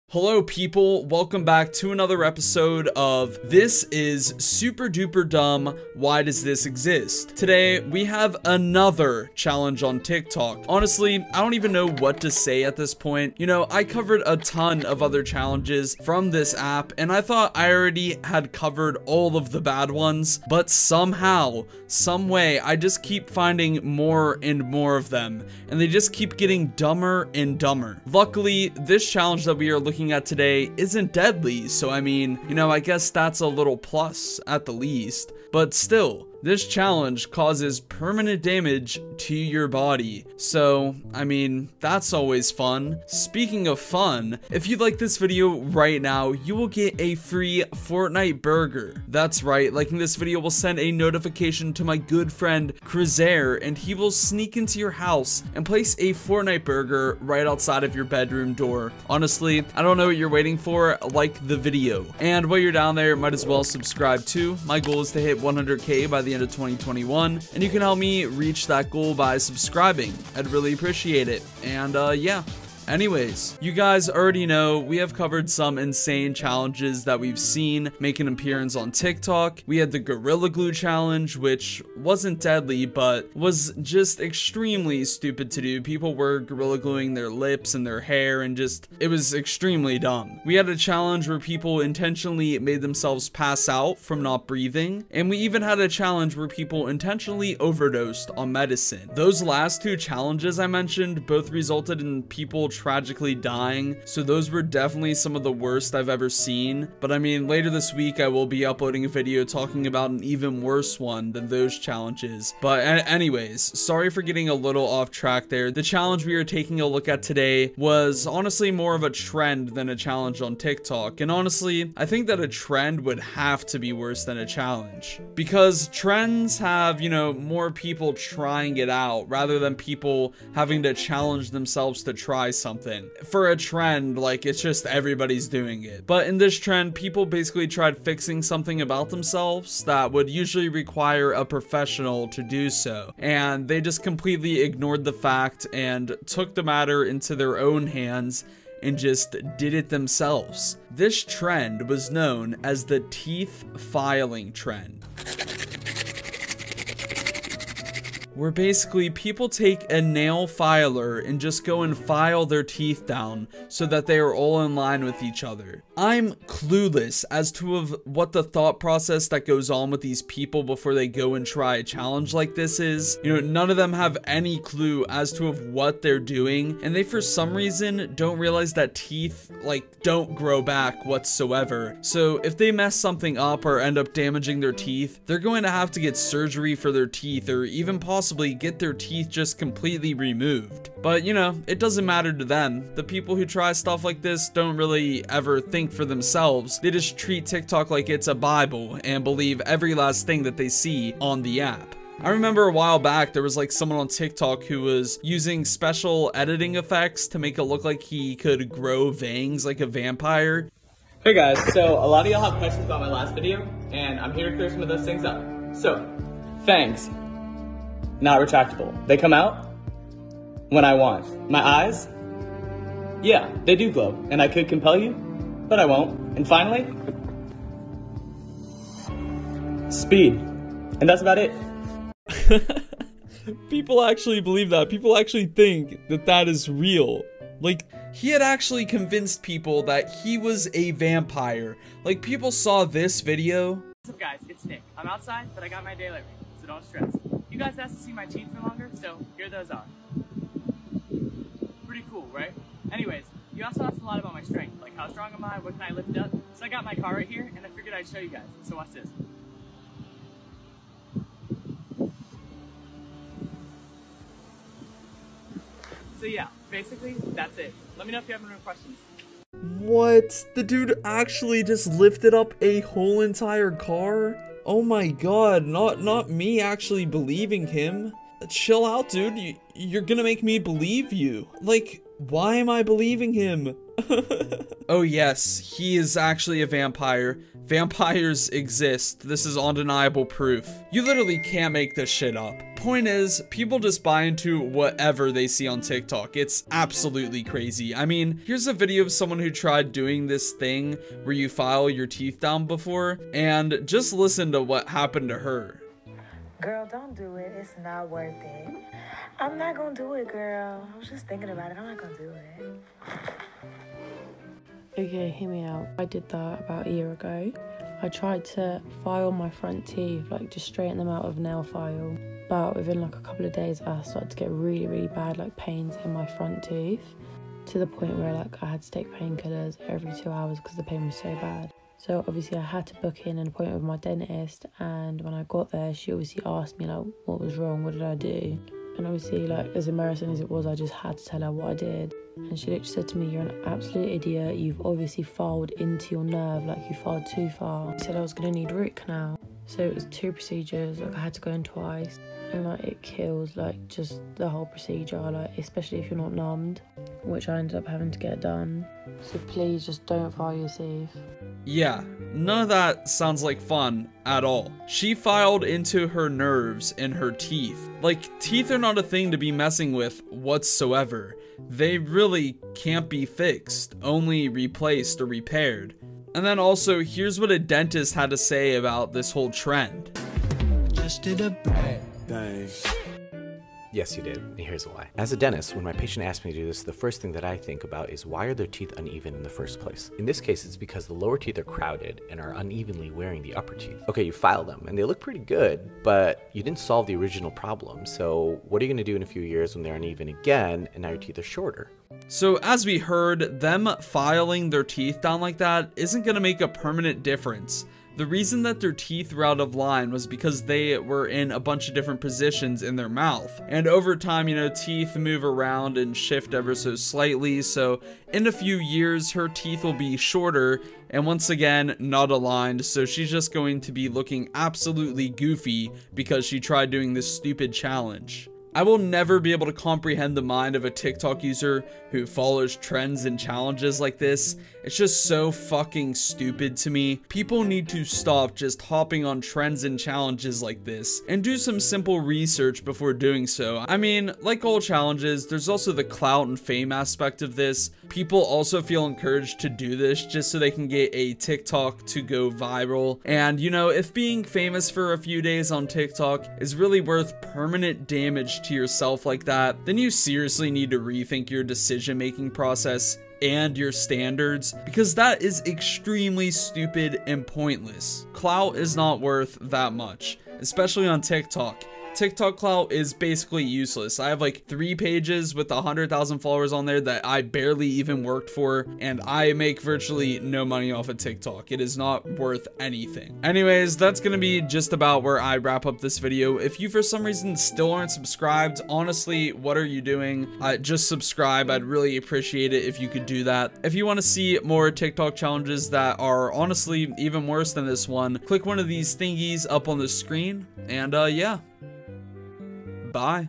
The DUMBEST TikTok Challenge To Exist...
The DUMBEST TikTok Challenge To Exist... This challenge has to be the dumbest yet... worst than "There is a gorilla glue challenge now???" even comparable to "The deadliest challenge on tiktok", and even "The most dangerous challenge on tiktok". The teeth filing challenge is so stupid and tik tok cringe
follow these to be cool person 😎

Huge shoutout to my channel members, thank you for your support :)